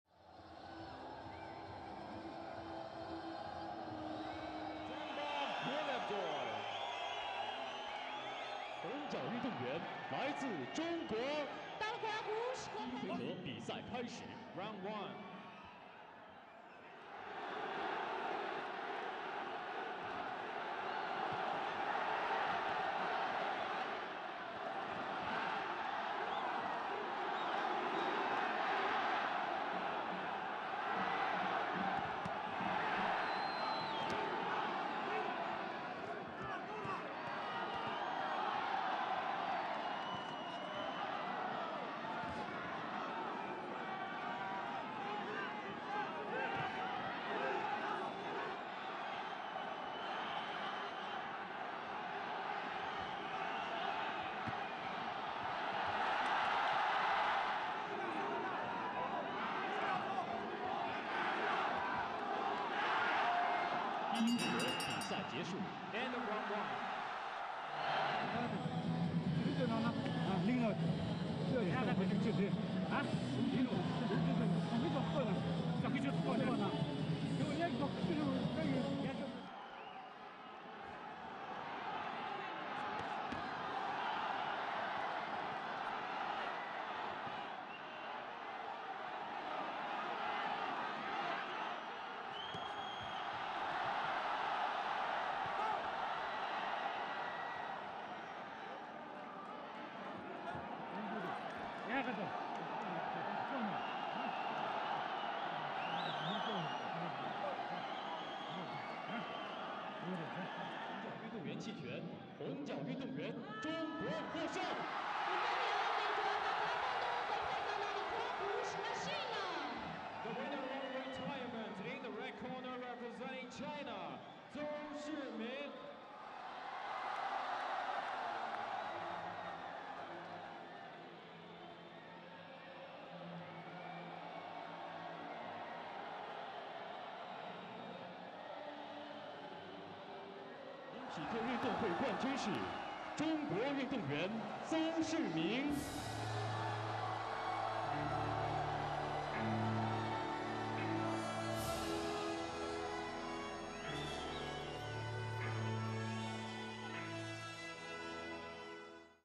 China vs Mongolia - Boxing - Light Flyweight 48KG - Beijing 2008 Summer Olympic Games
Shiming pulls out the big punches to score victory in the heavyweight division of the men's boxing at the Beijing 2008 Summer Olympic Games.